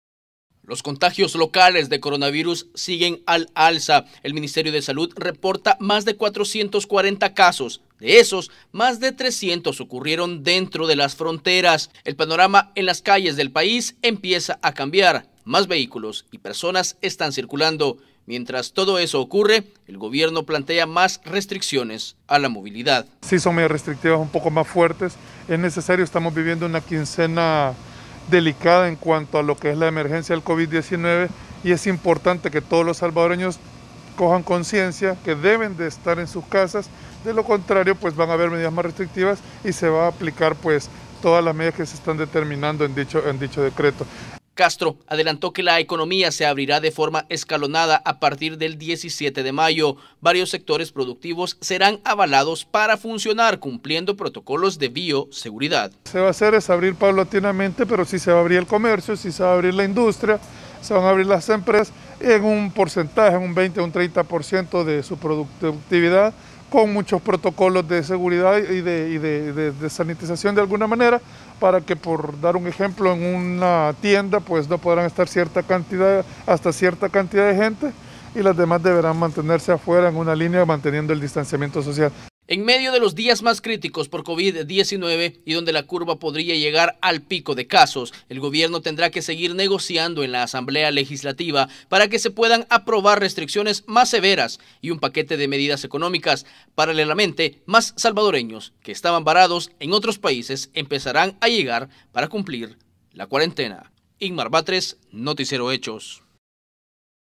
Gobierno plantea medidas más restrictivas a la movilidad de los salvadoreños
En El Salvador se enfrenta una quincena muy complicada debido a que los números de contagio alcanzarán el más alto, según las autoridades que buscan contener el contagio del covid-19.

¡Visítenos!